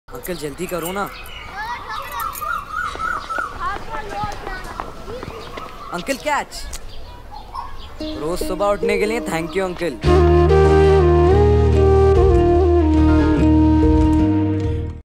Cadbury Dairy Milk - Playground - Hindi
Surely you've noticed, there are so many people who go about their jobs quietly, while making our lives easier and better all the time. Let's show them we know and remember to SayThankYou.